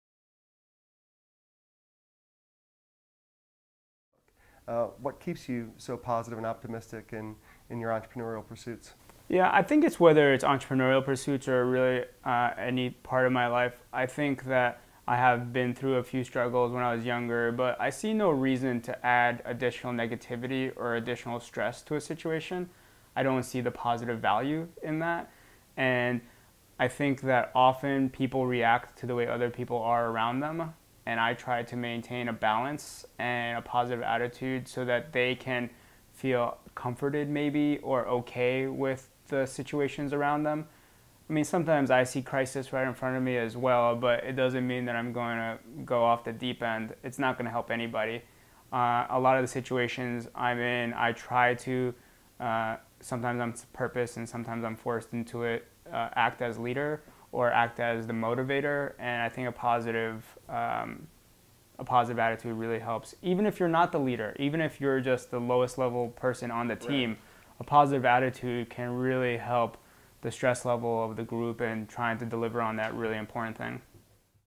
How to Lead Teams Using Optimism and Positive Attitude - Slava Rubin
In Chapter 8 of 16, entrepreneur Slava Rubin shares how childhood struggles and formative experiences have taught him to keep a balance and positive attitude. Rubin shares how positive energy and attitude - from the lowest level to highest level of the team - lower stress levels and raise team performance.